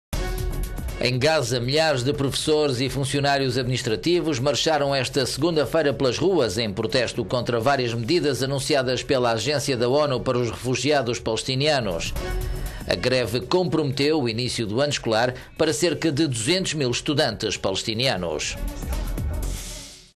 Gaza: professores descontentes atrasam início do ano escolar
Em Gaza milhares de professores e funcionários administrativos marcharam esta segunda-feira pelas ruas em protesto contra várias medidas anunciadas pela agência da ONU para os refugiados palestinianos, UNRWA.

A greve comprometeu o início do ano escolar para cerca de 200 mil estudantes palestinianos. Entre outras medidas, os professores reclamam contra o aumento do número de estudantes que podem chegar a meia centena por classe.